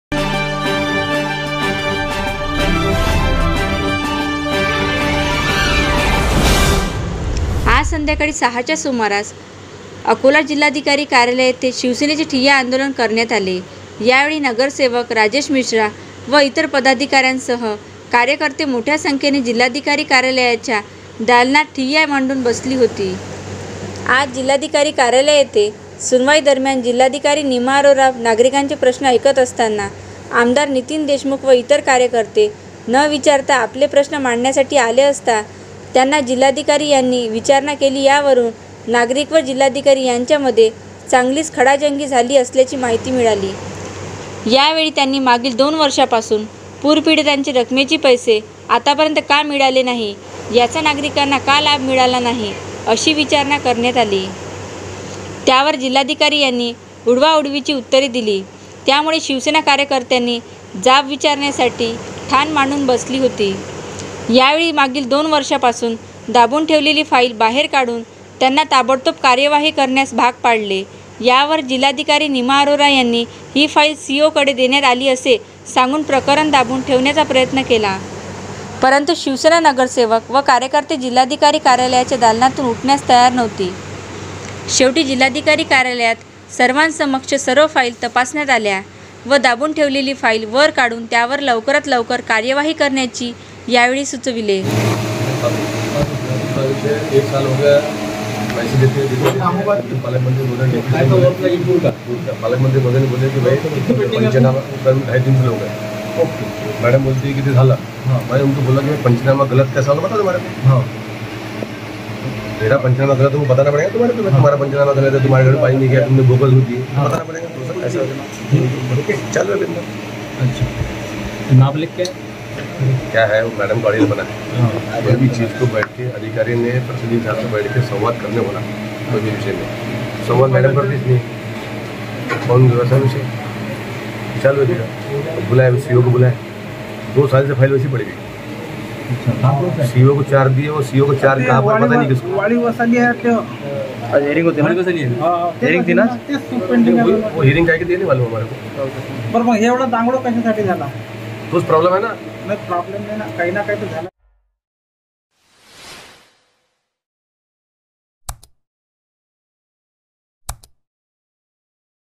शिवसेनेचे जिल्हाधिकारी दालनात ठिय्या आंदोलन
अकोला जिल्हाधिकार्यालयात शिवसेनेचे ठिय्या आंदोलन

आज संध्याकाळी सहाच्या सुमारास अकोला जिल्हाधिकारी कार्यालय येथे शिवसेनेचे ठिय्या आंदोलन करण्यात आले. यावेळी नगरसेवक राजेश मिश्रा व इतर पदाधिकाऱ्यांसह कार्यकर्ते मोठ्या संख्येने जिल्हाधिकारी कार्यालयाच्या दालनात ठिय्या मांडून बसली होती. आज जिल्हाधिकारी कार्यालय येथे सुनवाई दरम्यान जिल्हाधिकारी निमा अरोरा नागरिकांचे प्रश्न ऐकत असताना आमदार नितीन देशमुख व इतर कार्यकर्ते न विचारता आपले प्रश्न मांडण्यासाठी आले असता, त्यांना जिल्हाधिकारी यांनी विचारणा केली यावरून नागरीक व जिल्हाधिकारी यांच्यामध्ये चांगलीच खडाजंगी झाली असल्याची माहिती मिळाली.
यावेळी त्यांनी मागील दोन वर्षापासून पूरपीडितांचे रकमेची पैसे आतापर्यंत का मिळाले नाही, याचा नागरिकांना का लाभ मिळाला नाही? अशी विचारणा करण्यात आली. त्यावर जिल्हाधिकारी यांनी उडवा -उडवीची उत्तरे दिली, त्यामुळे शिवसेना कार्यकर्त्यांनी जाब विचारण्यासाठी ठान मांडून बसली होती. यावेळी मागील दोन वर्षापासून दाबून ठेवलेली फाईल बाहेर काढून त्यांना ताबडतोब कार्यवाही करण्यास भाग पाडले. यावर जिल्हाधिकारी निमा अरोरा यांनी ही फाईल सिओ कडे देण्यात आली असे सांगून प्रकरण दाबून ठेवण्याचा प्रयत्न केला. परंतु शिवसेना नगरसेवक व कार्यकर्ते जिल्हाधिकारी कार्यालयाच्या दालनातून उठण्यास तयार नव्हती. शेवटी जिल्हाधिकारी कार्यालयात सर्वान समक्ष सर्व फाईल तपासण्यात आला व दाबून धरली पाहिल वर काढून त्यावर लवकरात लवकर कार्यवाही करण्याची यावेळी सुचविले.